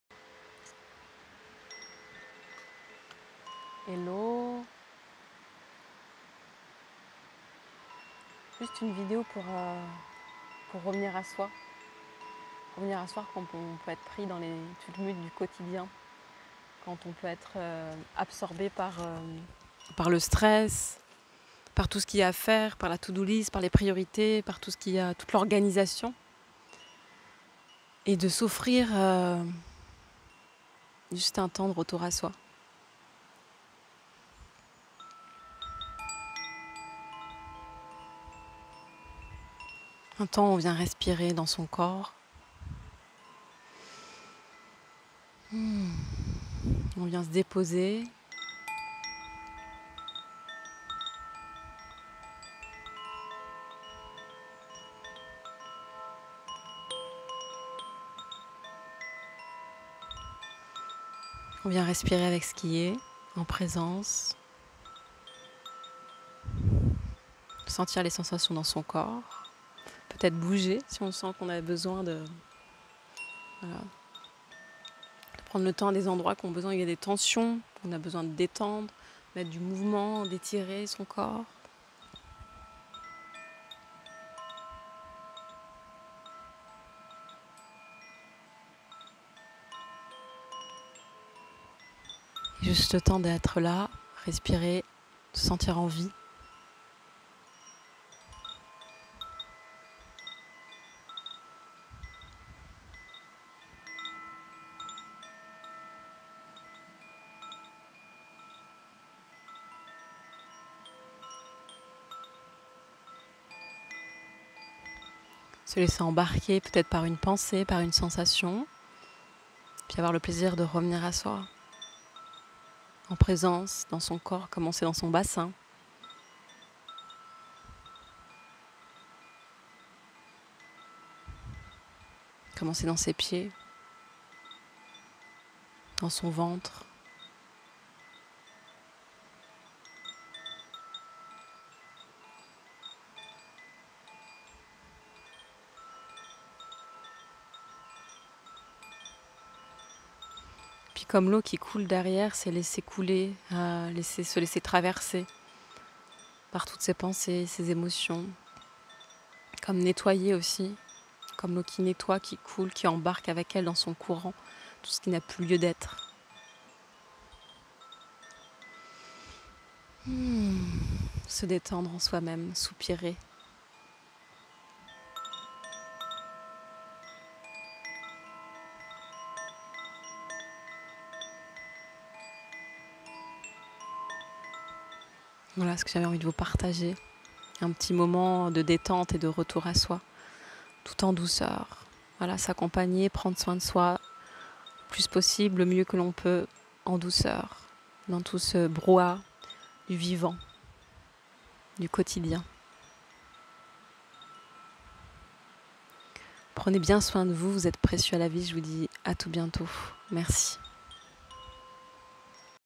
RETOUR A SOI
Juste respire, reviens à toi, et prends le temps.
Petit moment de détente.

Mathilde
Coach pour entrepreneurs et artistes